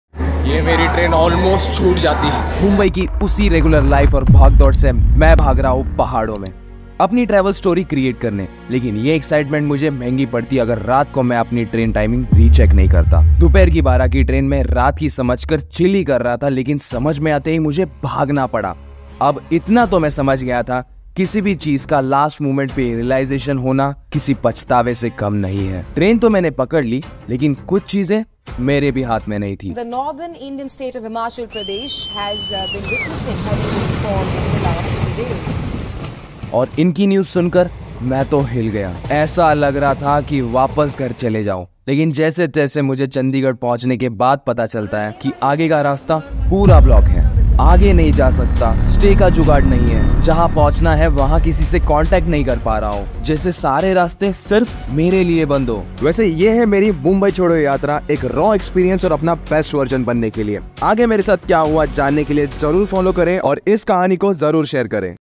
Phass Gaya? | Mumbai Choddo Yatra 01/69
Mai chodd Raha hu Mumbai kyuki yaha kuch ho nahi Raha!
Mai nikla hu ek raw experience aur apna best version Banne ke liye.
Ye hone wali hai meri 69 episodes ki series.
Self learning aur dedicated banne.
Keep supporting and loving❤️
1/69